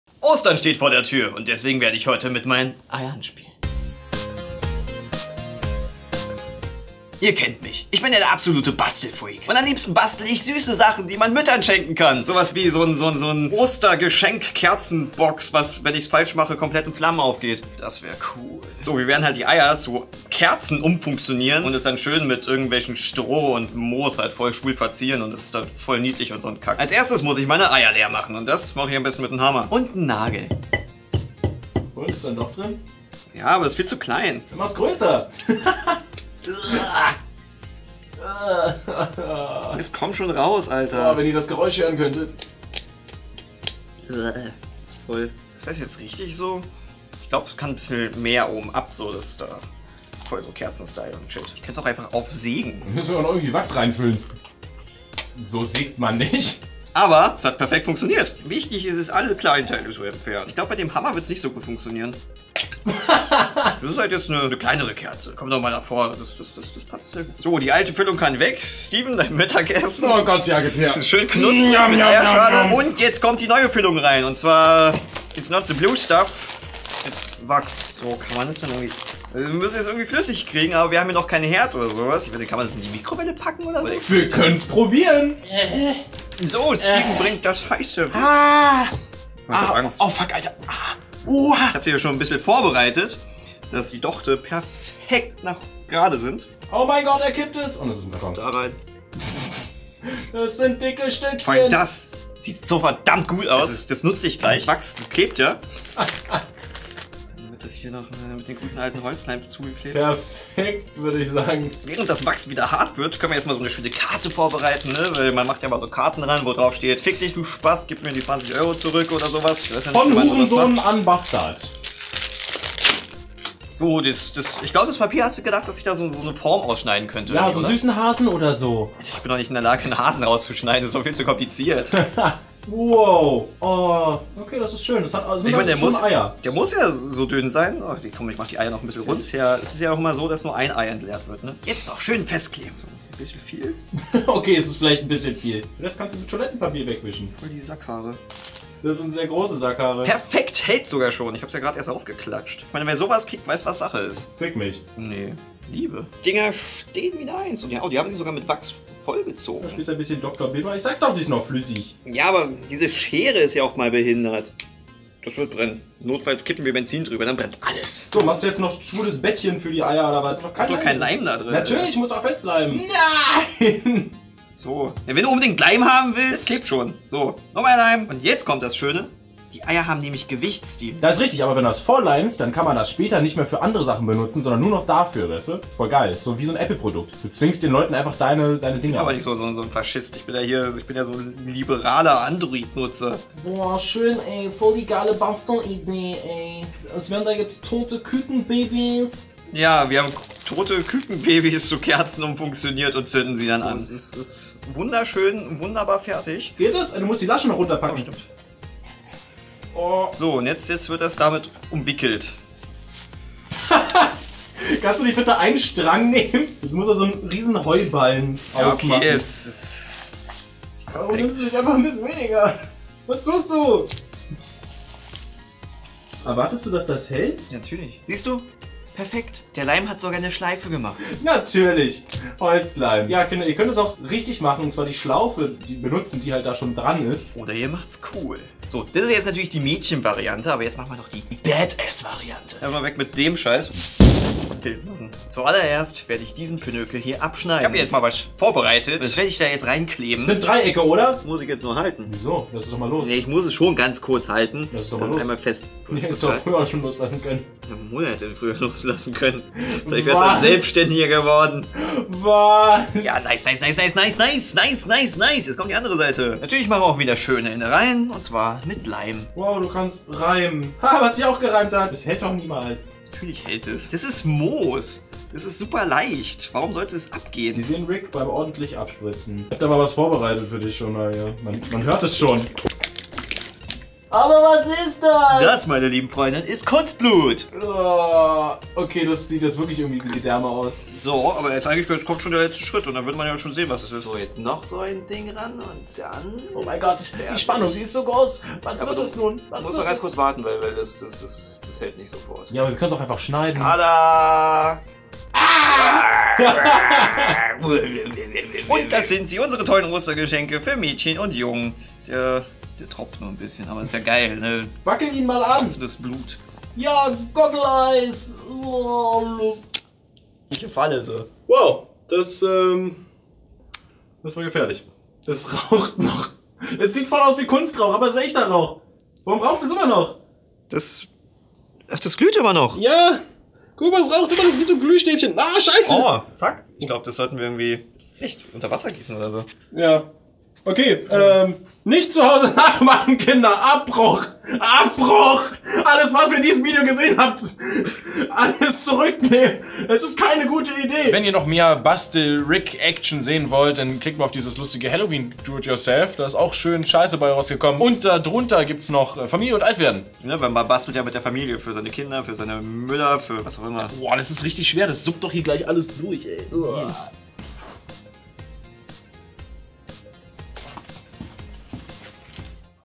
DIY Oster-Geschenkidee (Oder: Rick spielt mit seinen Eiern)
Da habta! Voll so das geile Ostern-Bastelvideo für die ganze Familie zum am besten nicht nachmachen, weil entflammbar. Bester Beschreibung von Video EVA.